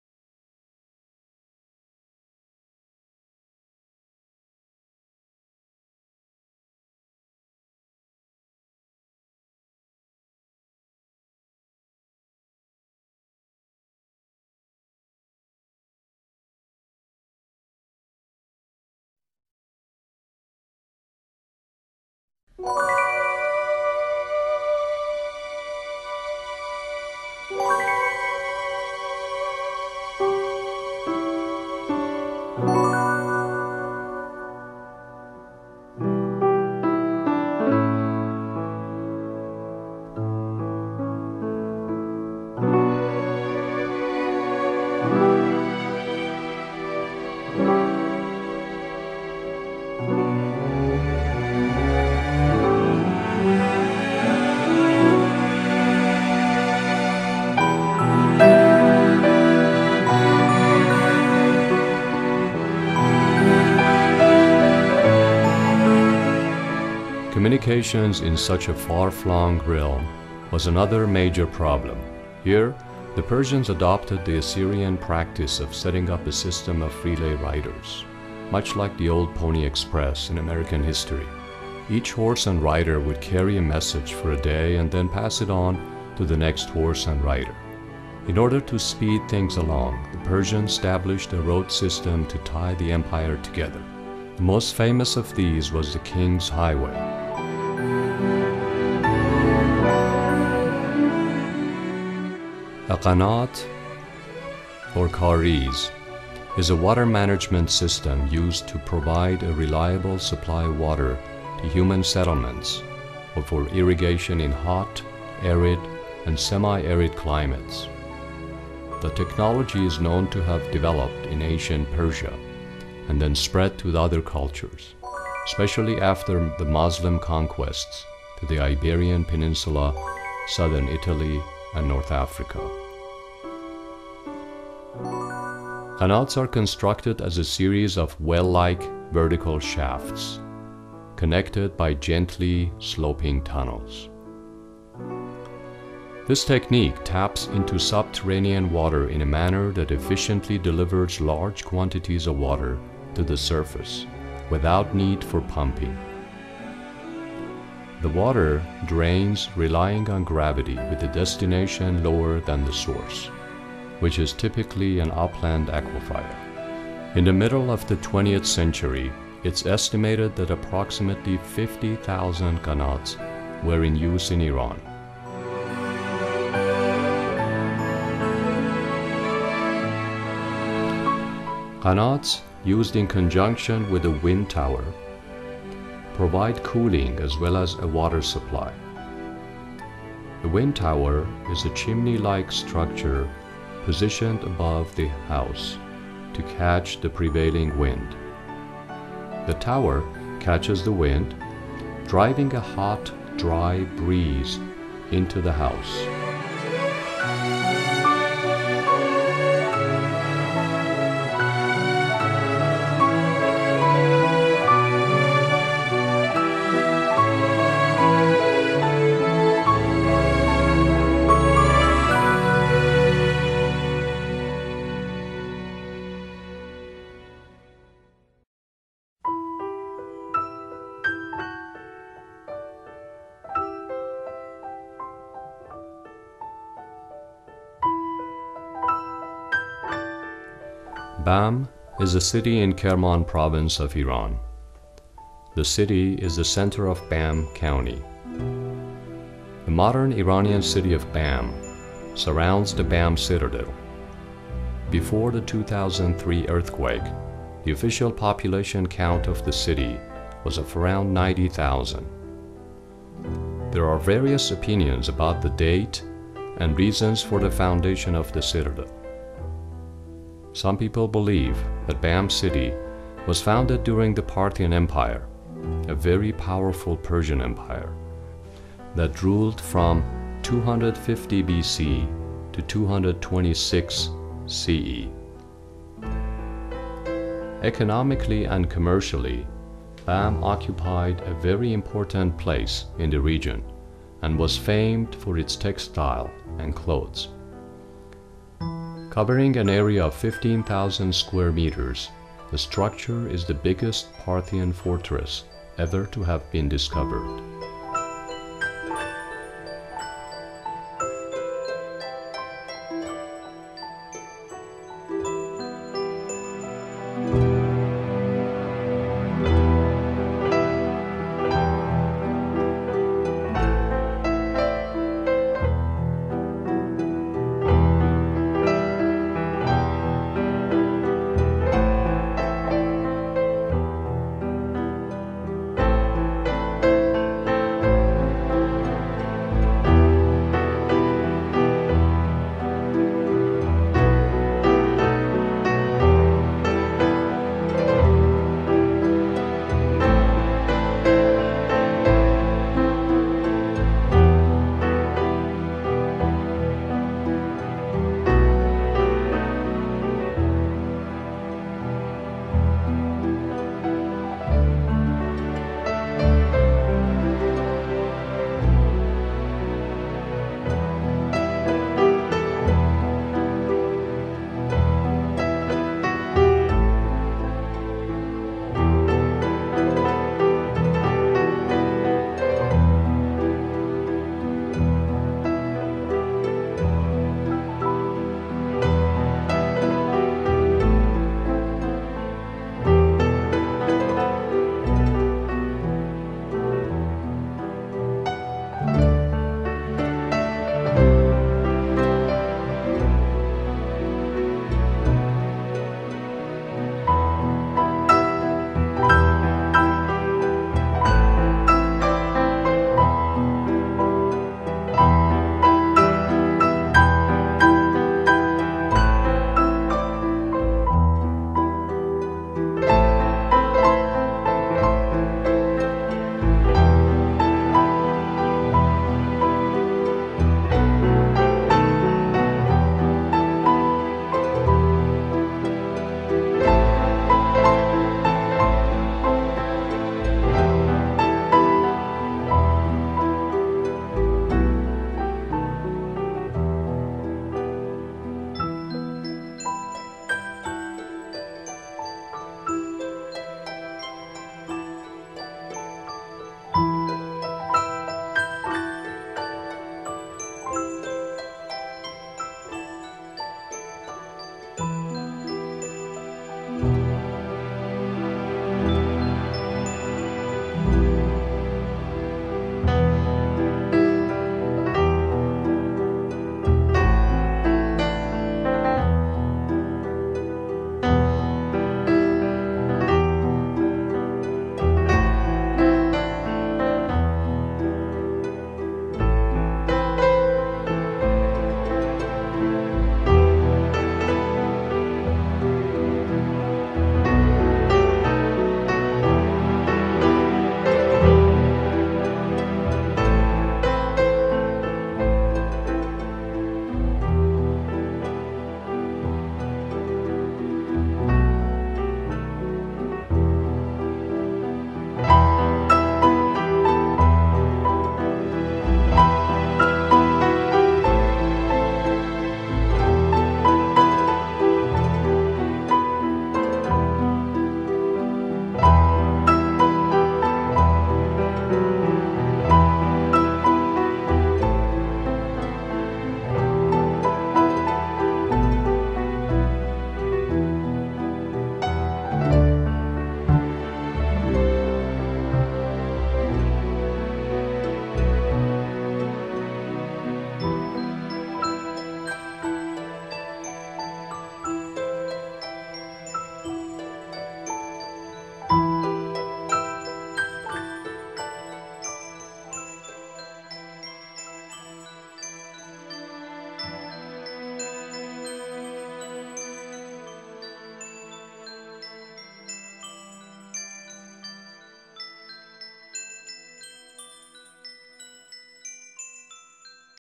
In English- Medes - Achaemenid - Parthian - Art and Technology Part IV Audio Overlay
Suez Canal, Ganats, Kahreez, natural air conditioning, floating bridges have been presented and narrated. Medes, Achaemenid and Parthian era.